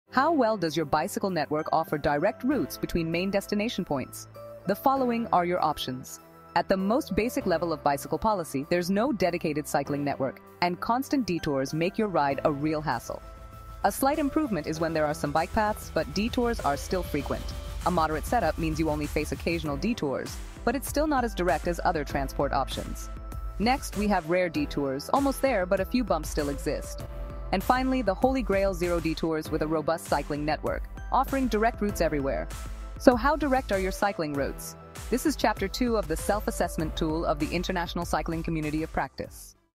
2. How well does the bicycle network offer direct routes between main destination points?
2. How well does the bicycle network offer direct routes between main destination points?

Constant Detours and Absence of a Dedicated Cycling Network:
This stage represents the most underdeveloped level of the cycling network in terms of offering direct routes to main destinations. Cyclists are forced to take constant detours, as there is an absence of a dedicated cycling network linking these key destinations. This situation often results in significantly longer and less convenient routes for cyclists compared to other transport networks. Such a lack of direct cycling routes not only discourages bicycle use but also highlights the pressing need for the development of a dedicated cycling network that provides efficient and direct connections to major destinations.

Frequent Detours with Occasional Cycling Network Presence:
At this level, while there is some presence of a cycling network linking main destinations, cyclists still face frequent detours. These detours are primarily due to the sporadic nature of the cycling network, which does not consistently cover all key routes. Compared to other transport networks, cyclists often find themselves on longer, less direct paths. Improving this situation requires expanding the cycling network to reduce the frequency of detours and provide more direct routes to major destinations.

Occasional Detours with Cycling Network Presence:
This stage indicates a moderate development of the cycling network, where cyclists encounter occasional detours but generally find a presence of cycling paths between main destinations. While this is an improvement over the previous levels, the network still lacks the comprehensiveness needed to provide consistently direct routes. Compared to other transport networks, the cycling routes are less direct, suggesting a need for further development and strategic planning to minimize detours and enhance the directness of the network.

Rare Detours with Cycling Network Presence:
At this advanced stage, the cycling network is well-developed, offering direct routes to main destinations with only rare occurrences of detours. This indicates a high level of integration of the cycling network with the overall transport infrastructure, allowing cyclists to travel efficiently and directly to key locations. To maintain and further enhance this level of connectivity, ongoing monitoring and refinements are necessary to ensure that new developments or changes in urban layout do not introduce new detours or disruptions in the network.

No Detours with Strong Cycling Network Presence:
This level represents the ideal state of a cycling network, where there are no detours, and a strong cycling network presence is evident between all main destinations. This reflects a scenario where the cycling network is fully integrated with the city’s transport infrastructure, providing cyclists with the most direct and efficient routes comparable to other modes of transport. Achieving and maintaining this state requires a continuous commitment to strategic planning, investment, and adaptation to evolving urban landscapes and transportation needs.